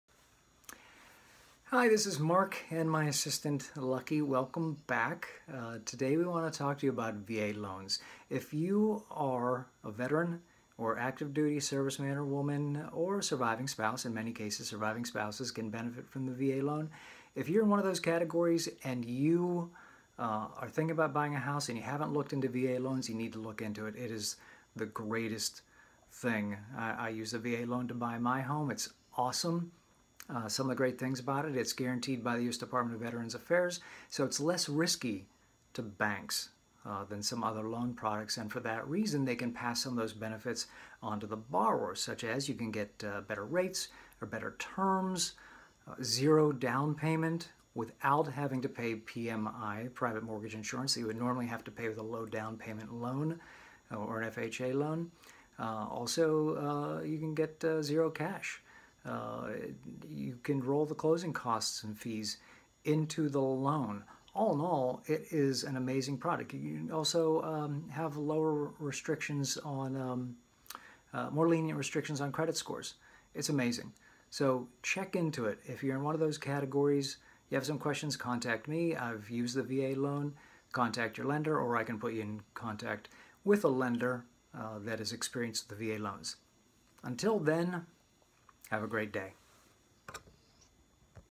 VA loans
Lower rates, better terms, no PMI, possible to have 0 down AND roll fees into the loan -- what's not to love?